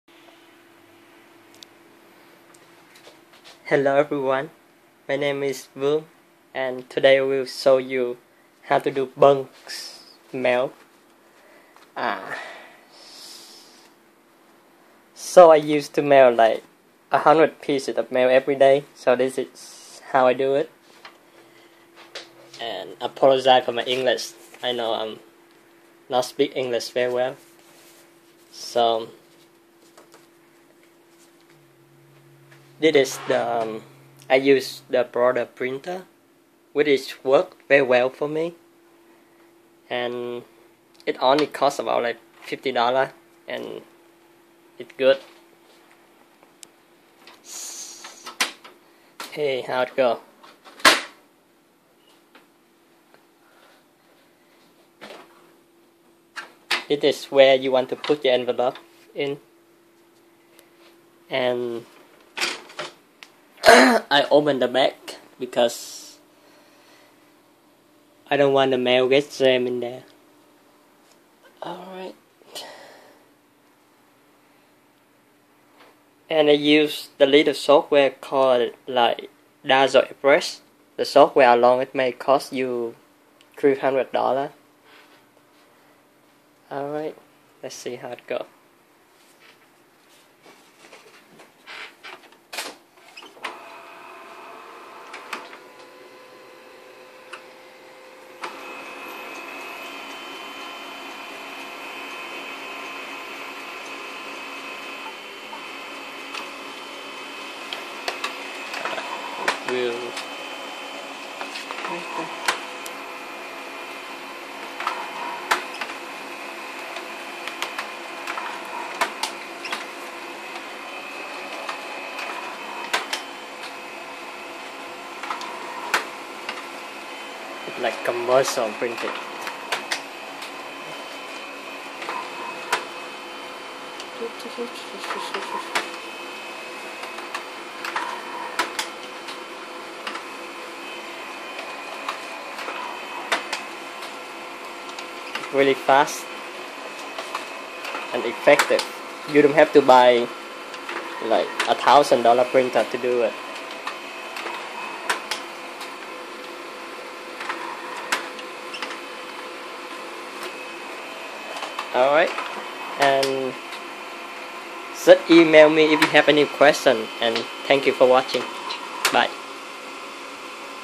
How to mass printing Presort standard envelope
Show you how do I mass printing hundred of presort standard mail envelopes